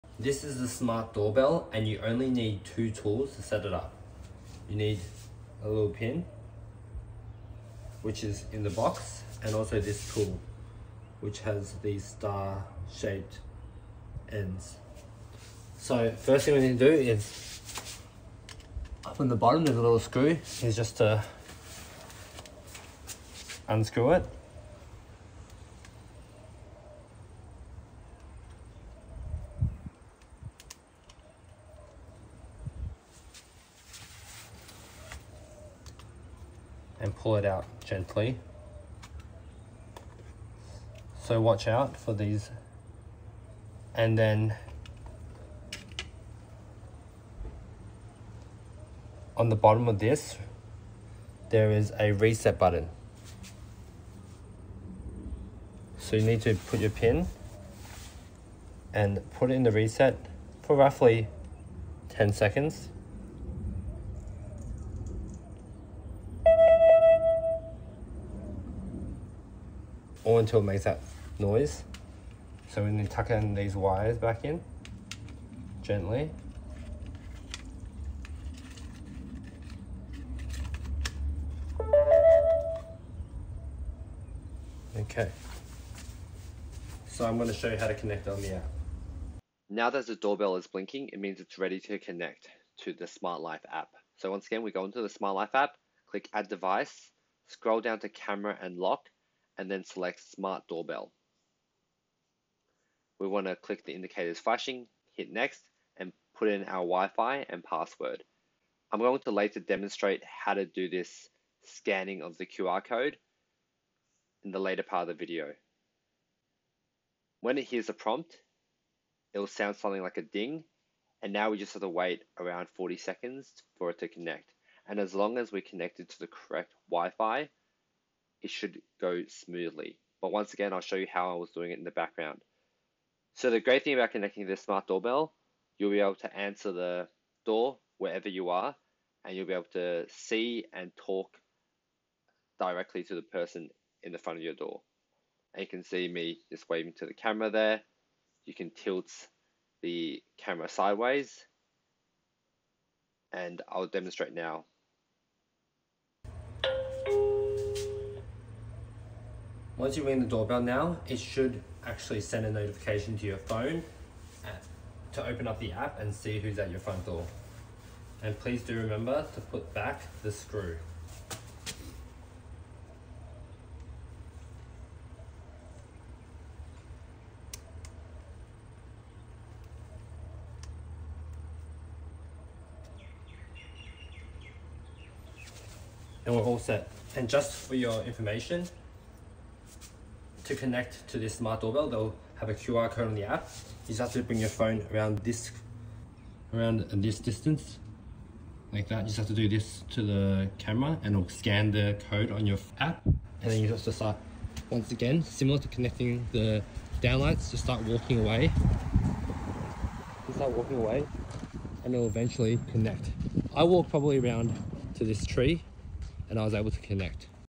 How to connect the Tuya Smart Doorbell to Smart Life App
Connecting Smart Tuya door bell to the Smart Life App.
Helpful tips for connecting Wi-Fi devices